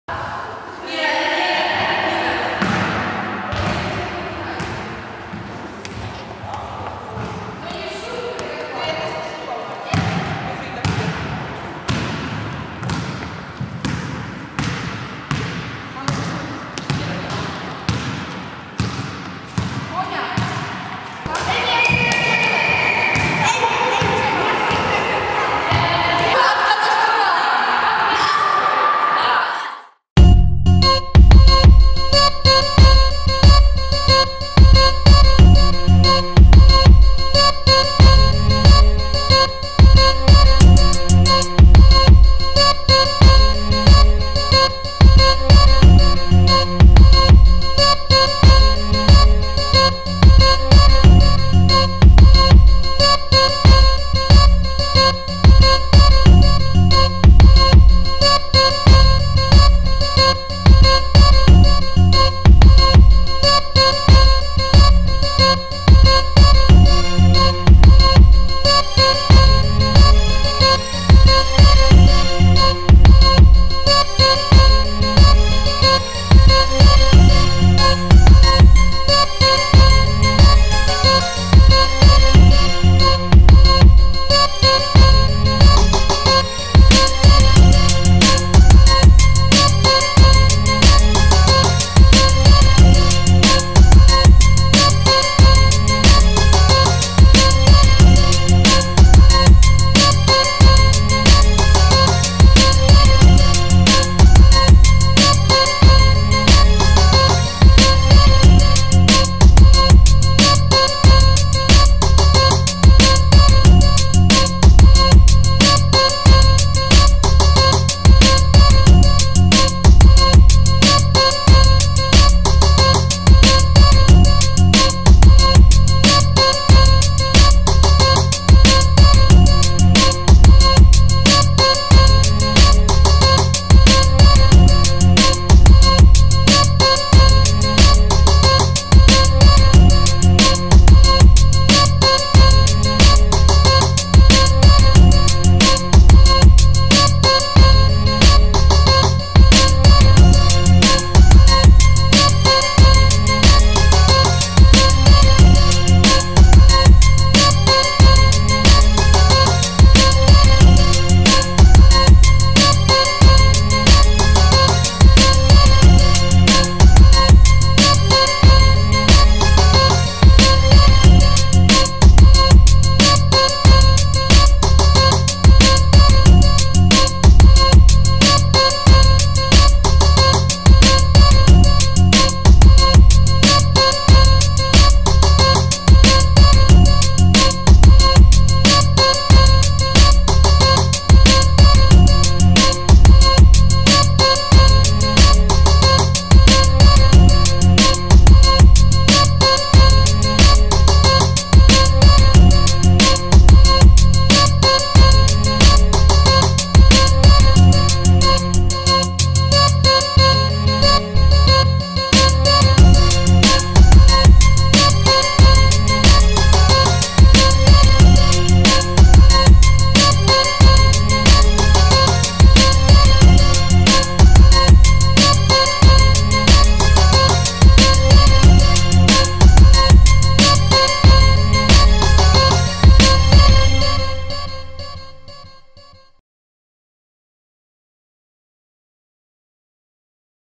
Zimowisko ZHP Mielec 2008r. - klip wideo, na sportowo
Zimowisko drużyn harcerskich
Hufca ZHP Mielec
w Przecławiu
19 - 23 stycznia 2008r.

klip wideo, na sportowo
 produkcja HarcFilm  ZHP Mielec